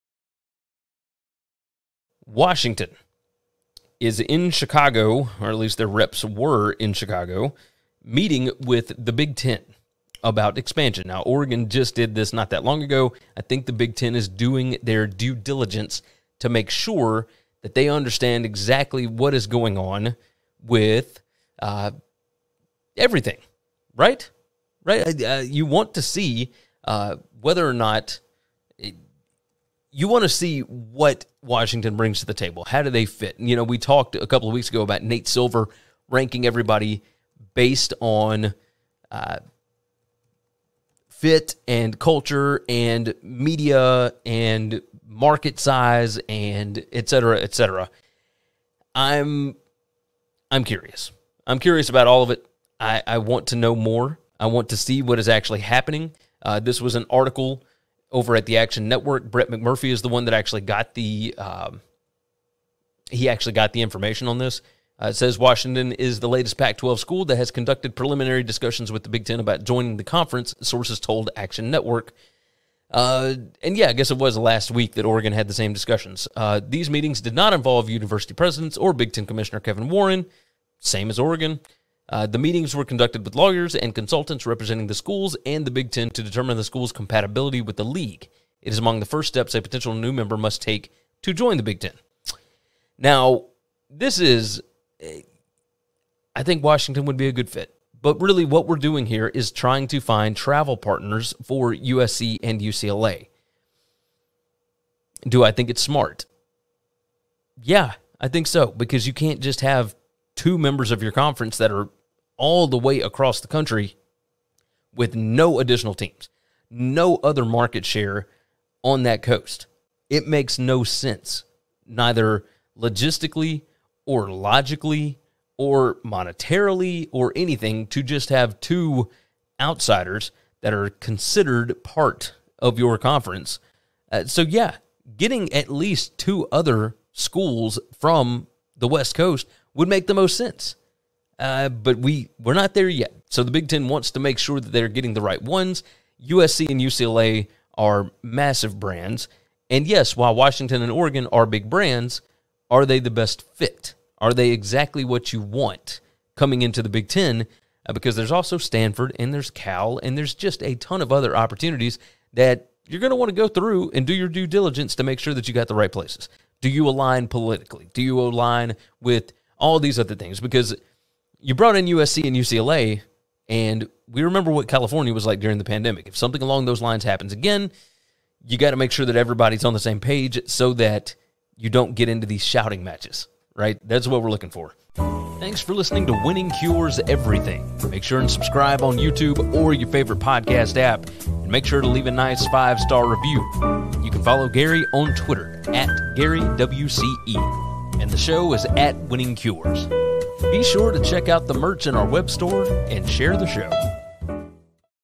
Washington & Big Ten meeting about expansion / realignment in Chicago
Brett McMurphy is reporting that Washington reps are in Chicago meeting with Big Ten reps about expansion, realignment, and the Huskies "fit" and "compatibility" with the Big 10 conference.

WCE is powered by BetUS!  BetUS.com